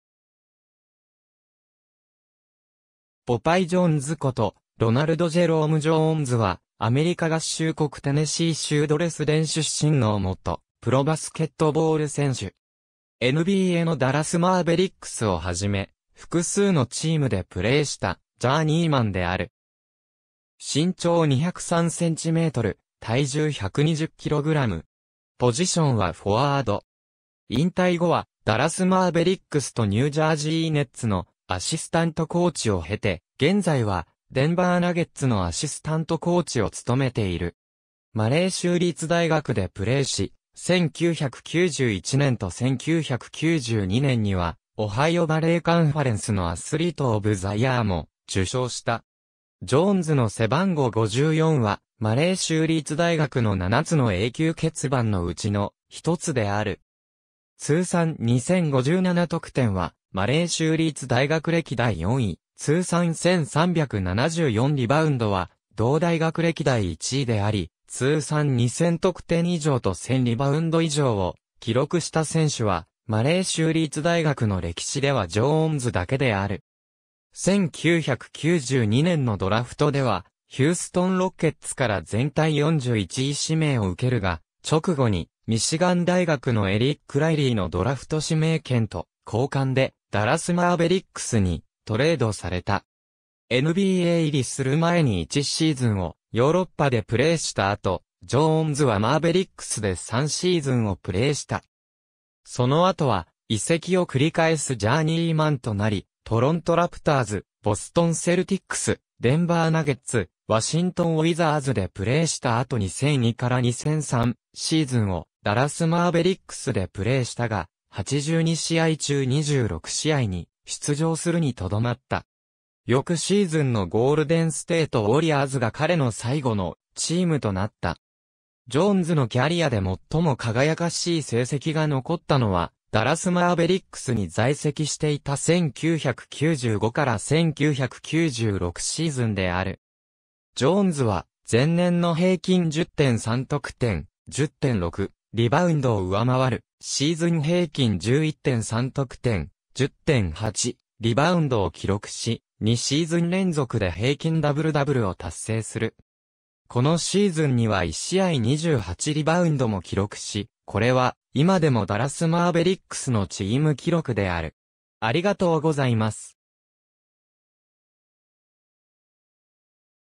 ポパイ・ジョーンズ
ポパイ・ジョーンズことロナルド・ジェローム・ジョーンズ（Ronald Jerome "Popeye" Jones, 1970年6月17日 - ）は、アメリカ合衆国テネシー州ドレスデン出身の元プロバスケットボール選手。
NBAのダラス・マーベリックスを始め、複数のチームでプレイしたジャーニーマンである。
引退後はダラス・マーベリックスとニュージャージー・ネッツのアシスタント・コーチを経て、現在はデンバー・ナゲッツのアシスタント・コーチを務めている。
マレー州立大学でプレイし、1991年と1992年にはオハイオ・バレー・カンファレンスのアスリート・オブ・ザ・イヤーも受賞した。
ジョーンズの背番号"54"はマレー州立大学の7つの永久欠番のうちの一つである。
通算2057得点はマレー州立大学歴代4位、通算1374リバウンドは同大学歴代1位であり、通算2000得点以上と1000リバウンド以上を記録した選手はマレー州立大学の歴史ではジョーンズだけである。
1992年のドラフトではヒューストン・ロケッツから全体41位指名を受けるが、直後にミシガン大学のエリック・ライリーのドラフト指名権と交換でダラス・マーベリックスにトレードされた。
NBA入りする前に1シーズンをヨーロッパでプレイした後、ジョーンズはマーベリックスで3シーズンをプレイした。
その後は移籍を繰り返すジャーニーマンとなり、トロント・ラプターズ、ボストン・セルティックス、デンバー・ナゲッツ、ワシントン・ウィザーズでプレイした後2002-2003シーズンをダラス・マーベリックスでプレイしたが、82試合中26試合に出場するにとどまった。
翌シーズンのゴールデンステート・ウォリアーズが彼の最後のチームとなった。
ジョーンズのキャリアで最も輝かしい成績が残ったのはダラス・マーベリックスに在籍していた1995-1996シーズンである。
ジョーンズは前年の平均10.3得点、10.6リバウンドを上回るシーズン平均11.3得点、10.8リバウンドを記録し、2シーズン連続で平均ダブル・ダブルを達成する。
このシーズンには1試合28リバウンドも記録し、これは今でもダラス・マーベリックスのチーム記録である。